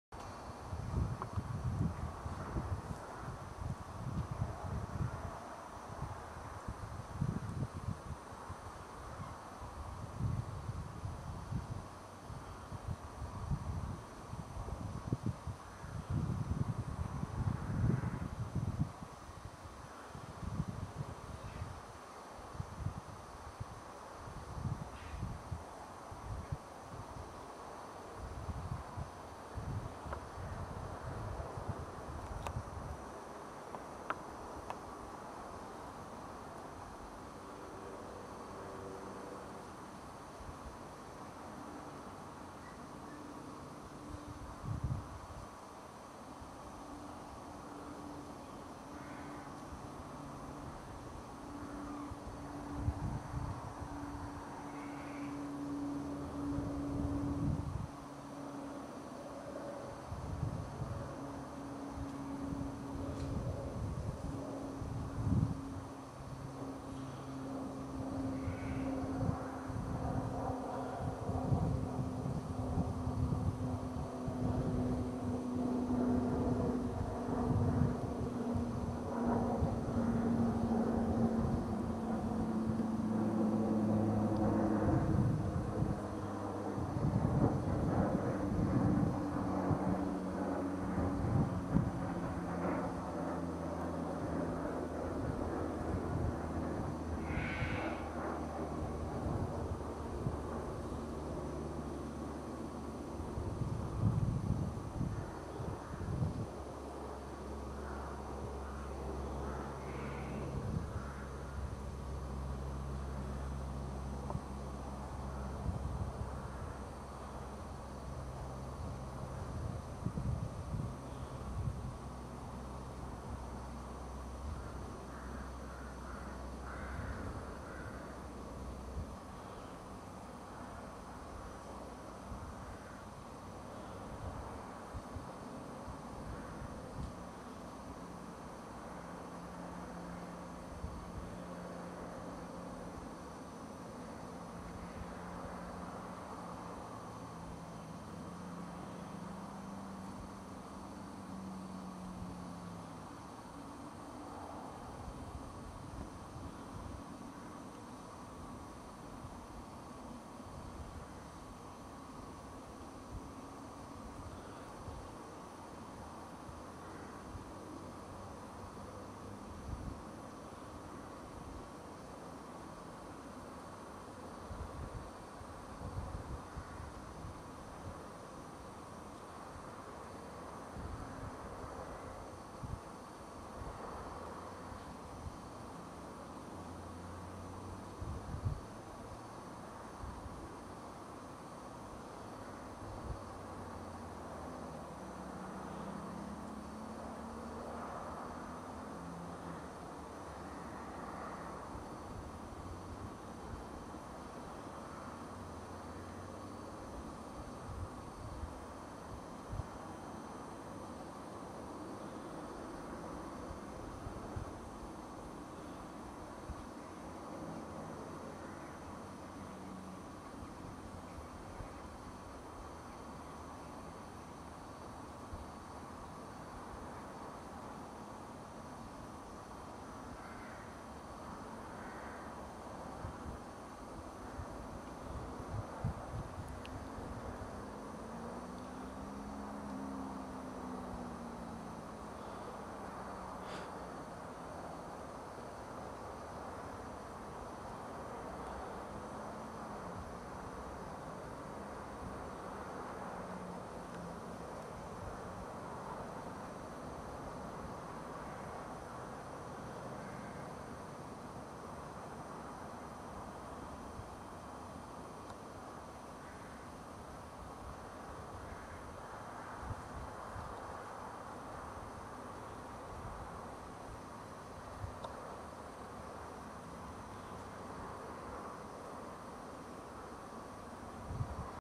Wild Scottish Fox: looking for breakfast in a sheep pasture.
Is he after mutton? Not a bit of it, the fox takes no notice of the sheep and the sheep take no notice of the fox.  Breakfast is evidently much much smaller.    Good close view towards the end. Ambient sound.